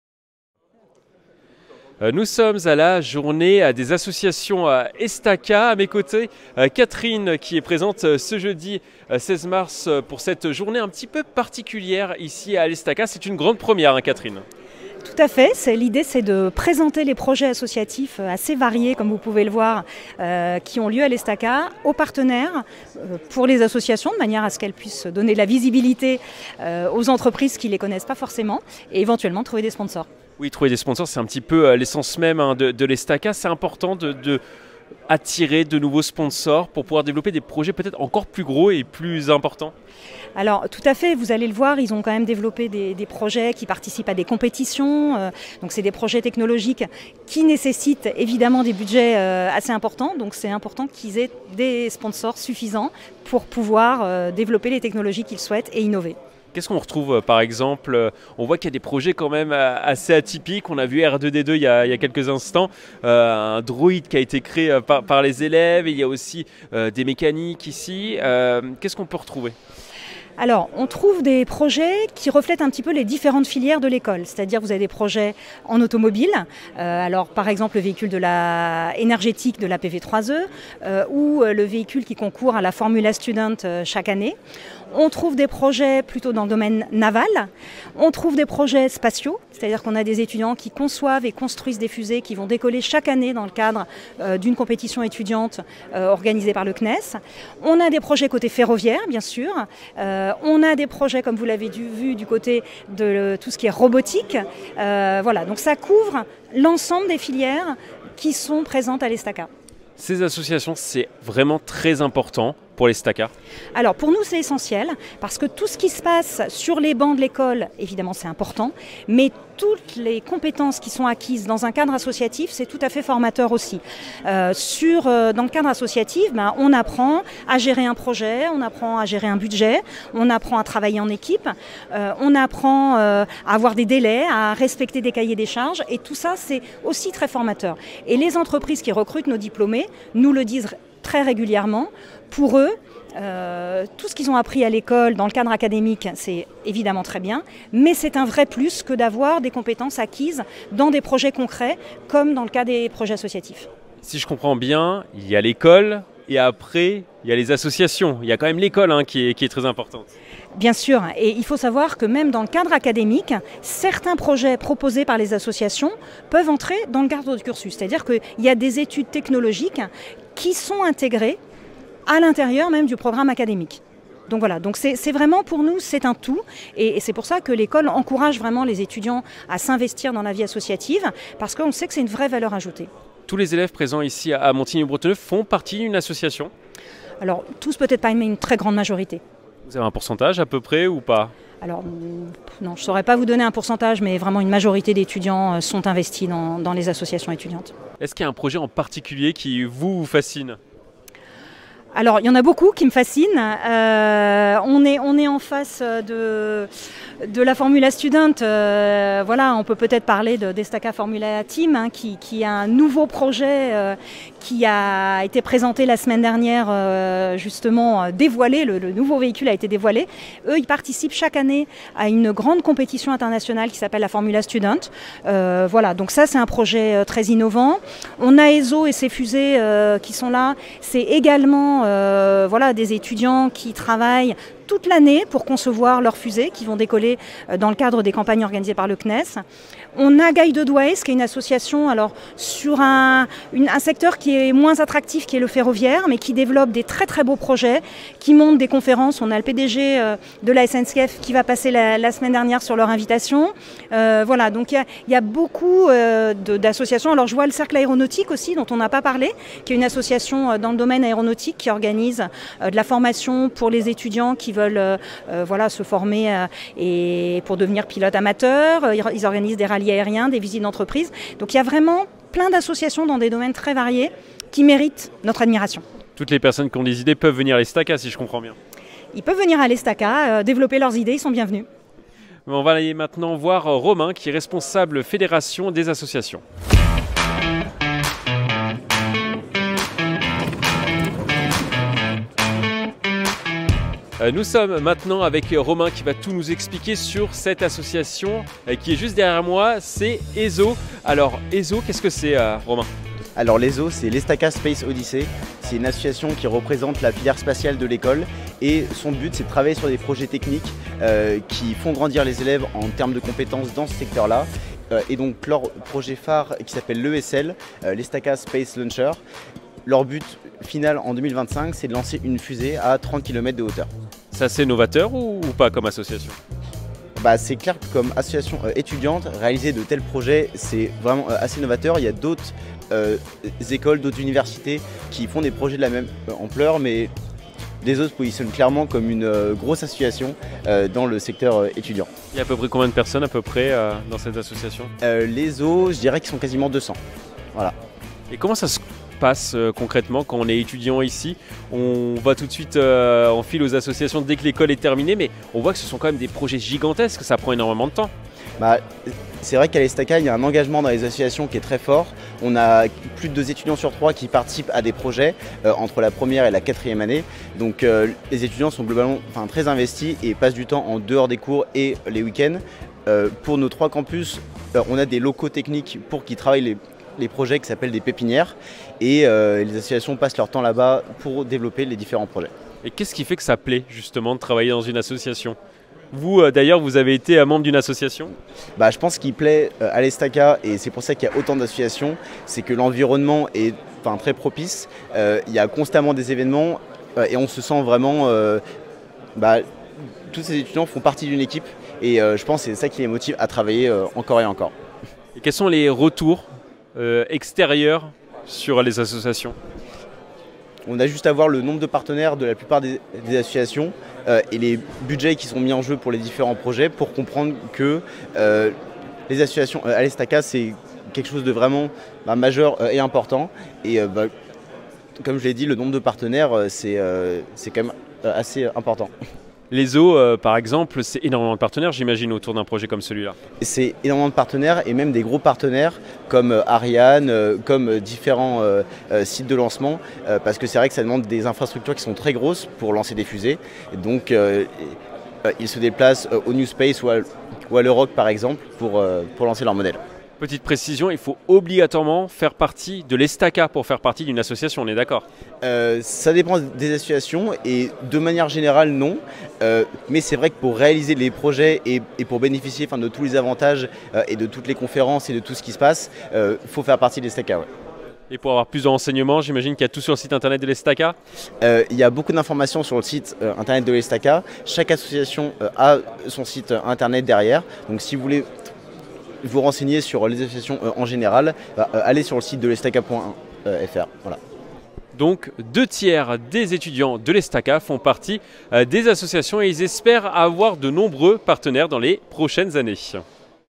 L'école d'ingénieurs « Estaca » présente ses projets associatifs
Le 16 mars dernier, l'école d'ingénieurs "Estaca" a présenté les projets associatifs de ses étudiants. Des travaux qui concernent la mobilité du futur.